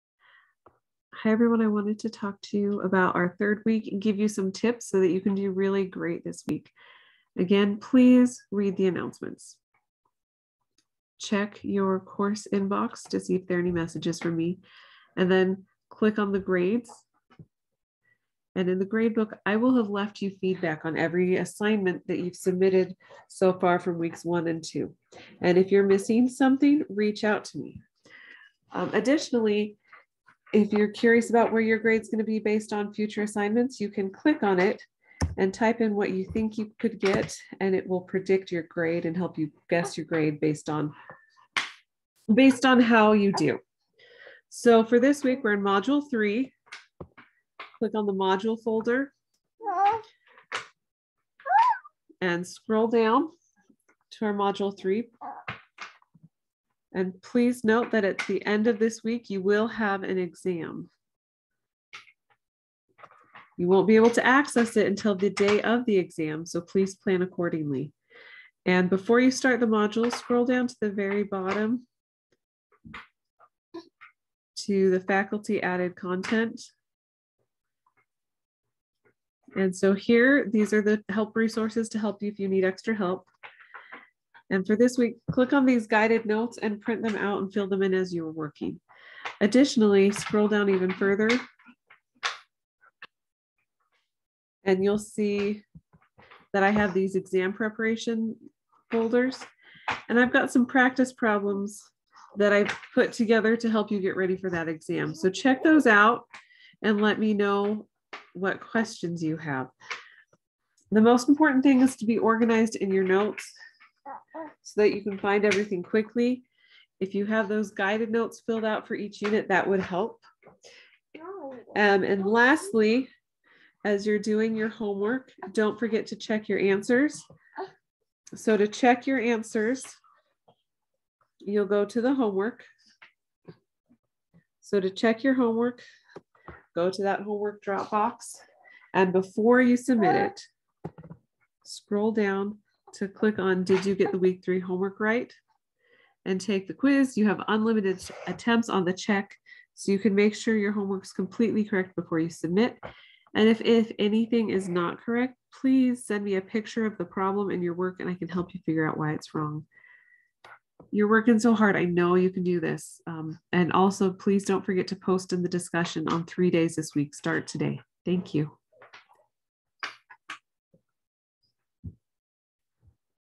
Galen Week 3: Math 1205
This video will walk you through week 3 and help you have ideas on how to prepare for the exam.